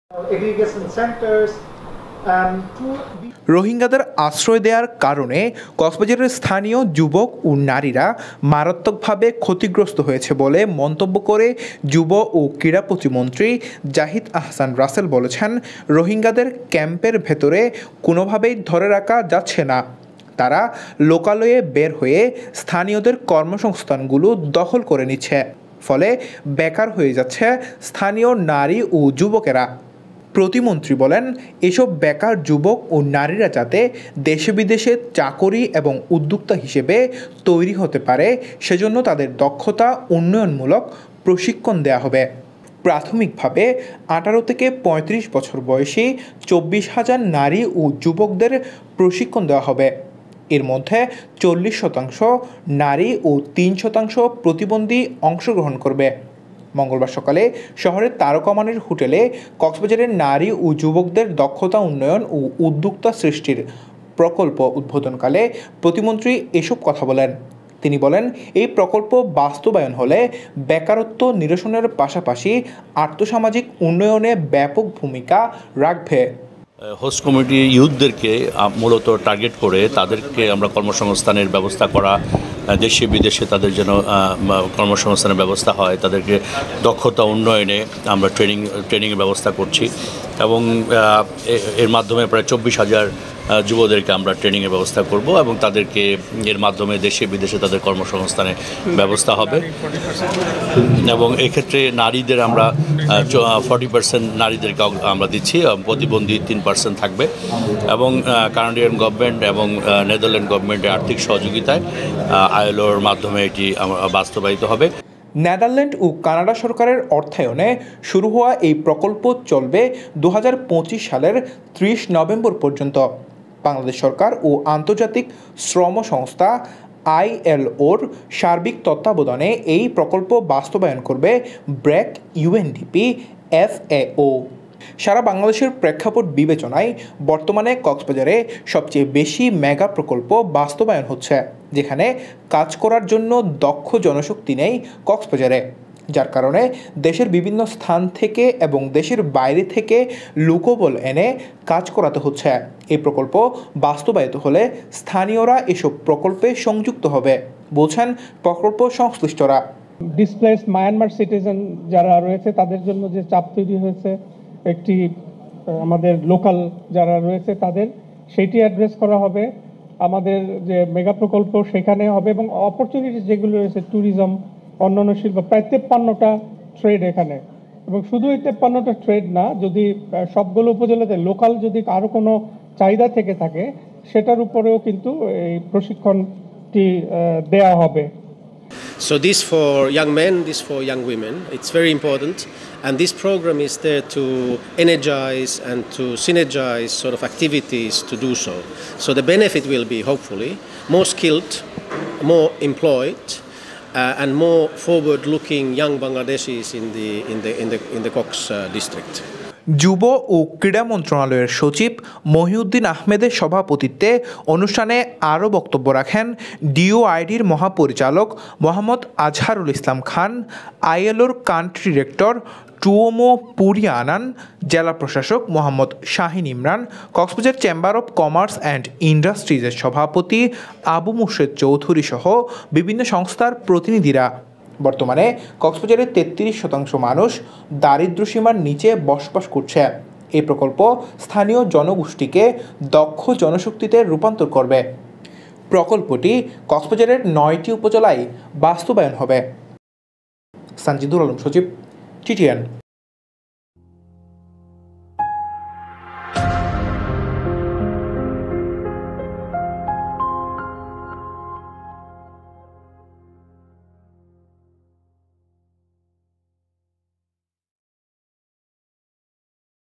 -ttn কক্সবাজারের ২৪ হাজার নারী পুরুষকে স্বাবলম্বী করা হবে-যুব ও ক্রীড়া প্রতিমন্ত্রী রাসেল
কক্সবাজারের ২৪ হাজার নারী পুরুষকে স্বাবলম্বী করা হবে-যুব ও ক্রীড়া প্রতিমন্ত্রী রাসেল

Slogan: বস্তুনিষ্ঠ সংবাদ সবার আগে
Country: Bangladesh
Language: Bengali।English
Broadcast area: Online